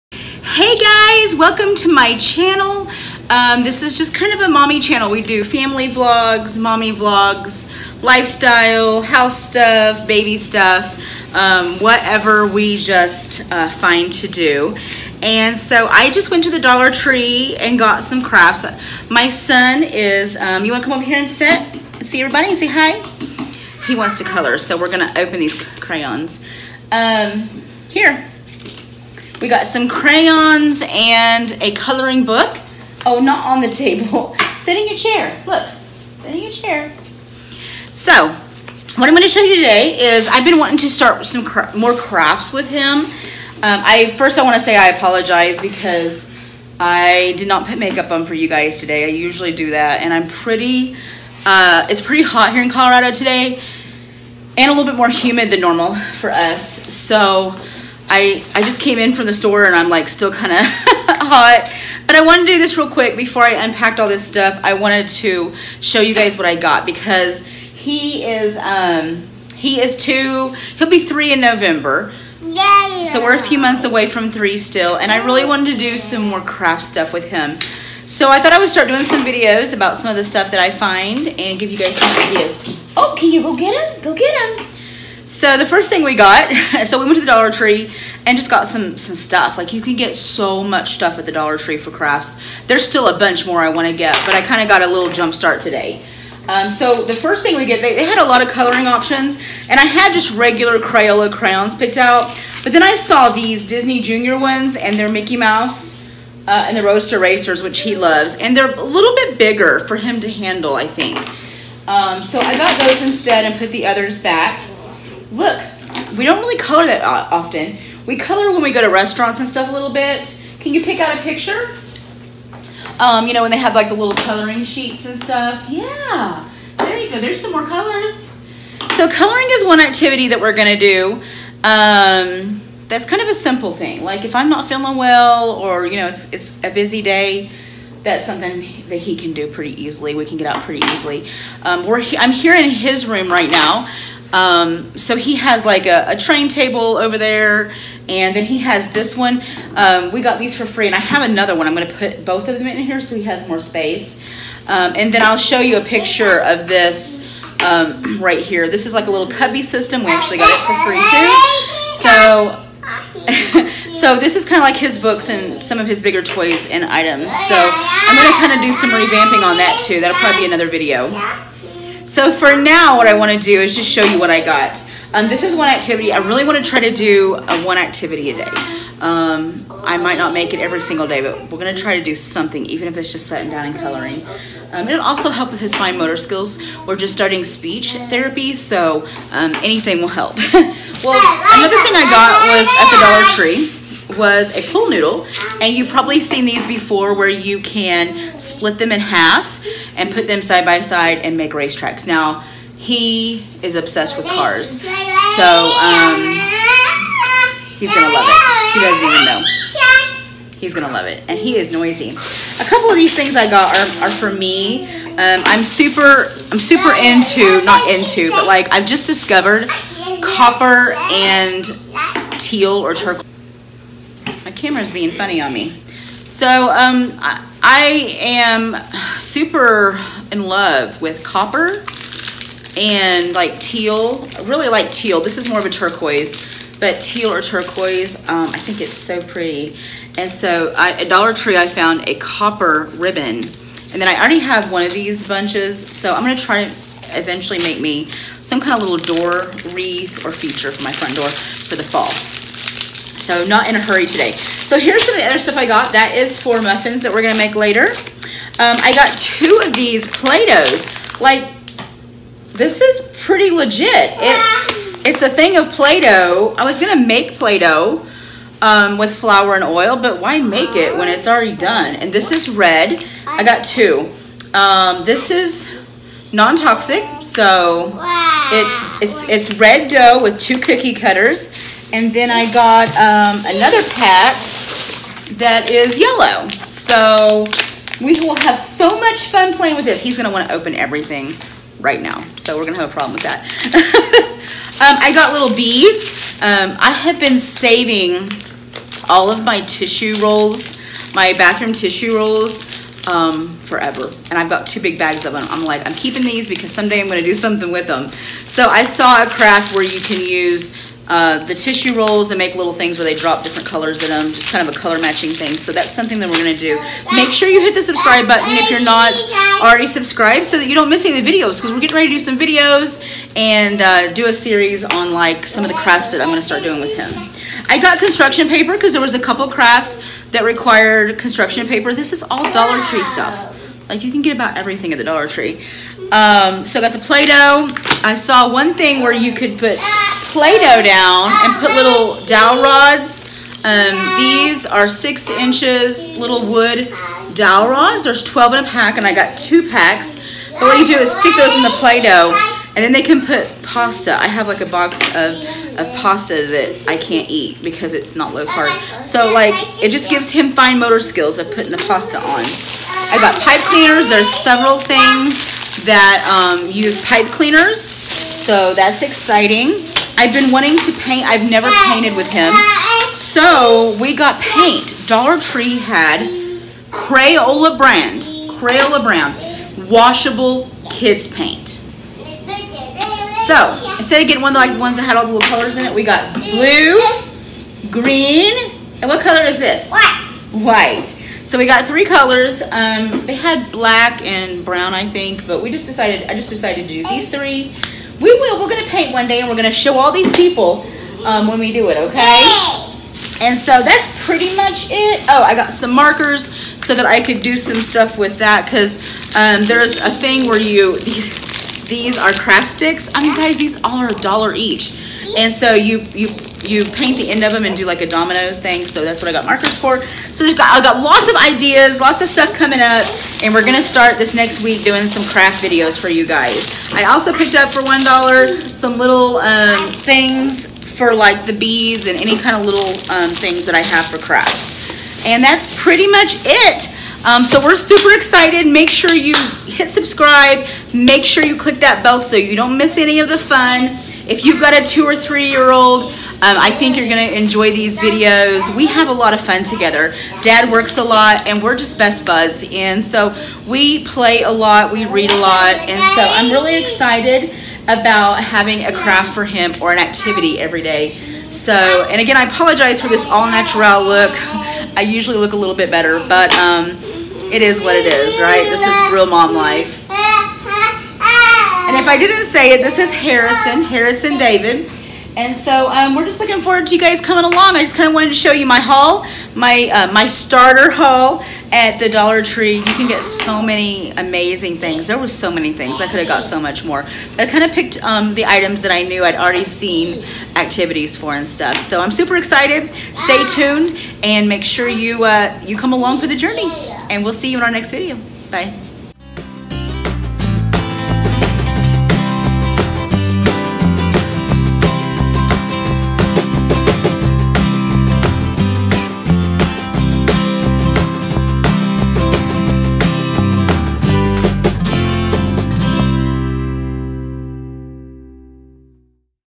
DOLLAR TREE CRAFT HAUL | TODDLER CRAFTS AND ACTIVITIES
This is my starter haul for Harrison's crafts and activities!